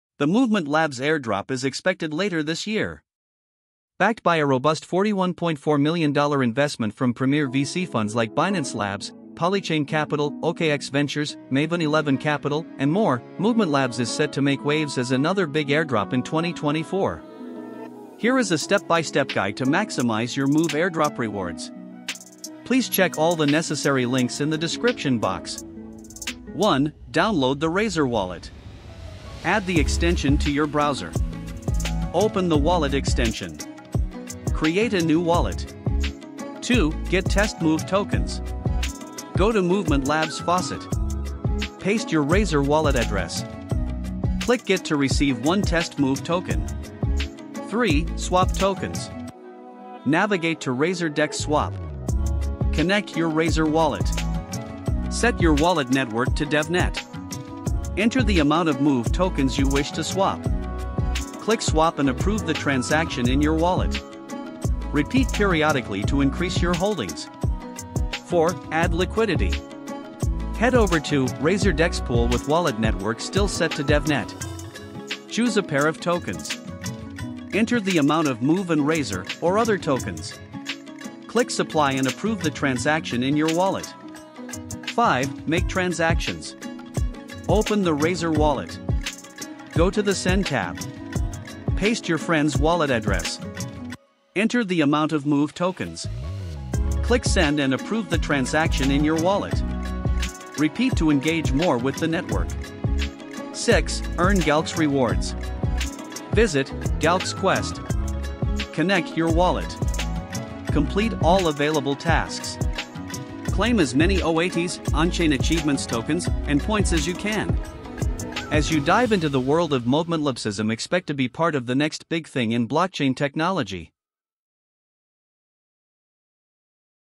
How to Unlock Massive Rewards in The Upcoming $MOVE Airdrop🪂
Guide to Unlocking Massive $MOVE Airdrop

1. **Download the Razor Wallet:**
   - Download and install the extension.
   - Open the wallet extension and create a new wallet.

2. **Get Faucet Tokens:**
   - Paste your Razor wallet address.
   - Click on the "Get" button to receive 1 test $MOVE token.

3. **Perform Swaps:**
   - Connect your Razor wallet.
   - Set network to DevNet
   - Enter the amount of $MOVE tokens.
   - Click the "Swap" button and approve the transaction in your wallet.
   - Repeat this process periodically.

4. **Add Liquidity:**
   - Select a pair of tokens.
   - Enter the amounts of $MOVE and $RAZOR tokens (for example).
   - Click the "Supply" button and approve the transaction in your wallet.

5. **Make Transactions:**
   - Open the Razor wallet.
   - Navigate to the "Send" tab.
   - Paste your friend's wallet address.
   - Enter the amount of $MOVE tokens (for example).
   - Click the "Send" button and approve the transaction in your wallet.

6. **Complete Galxe Quests:**
   - Connect your wallet.
   - Complete all available tasks.
   - Claim as many OATs and points as possible.

Enjoy using your Razor wallet!